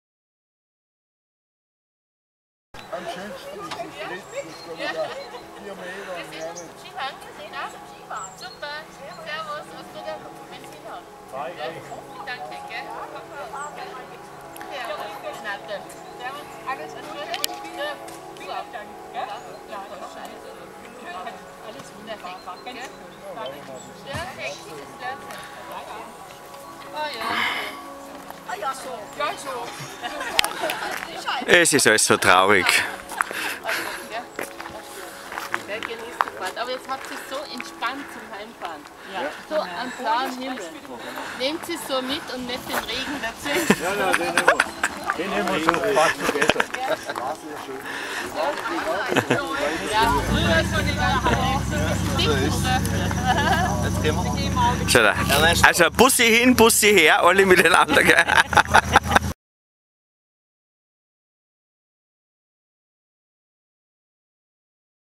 Morgan Ausfahrt vom Schloss Thannegg
Das Schloss Thannegg ist der Treffpunkt für Morgan Car Fans aus ganz Europa. Schlossherr Ernst - selbst leidenschaftlicher Morgan Fahrer - begleitet seine Gäste auch persönlich auf den schönsten Touren des Ennstal-Classic-Roadbooks.
Dieses Video zeigt die Teilnehmer bei der Abreise bei Kaiserwetter.